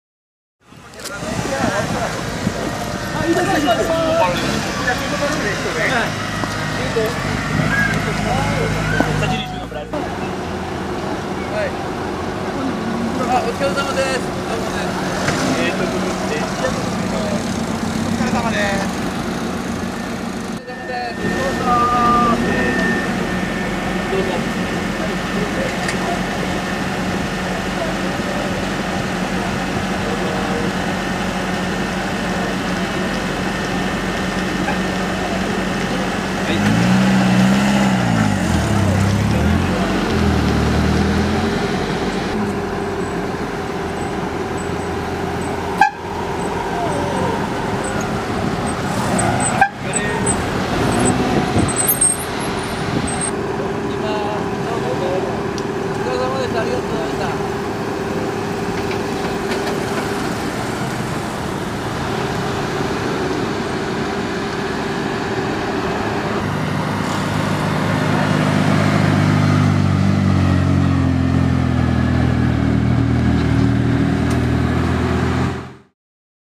【 Motor Sports Japan Festival 2013 】退出する6台のWRCレプリカ・カー Pt1【 World Rally Championship 】
2013年10月14日、お台場特設会場で開催されたモータースポーツジャパン2013フェスティバルに展示されたWRCレプリカ・カーが退出する様子です。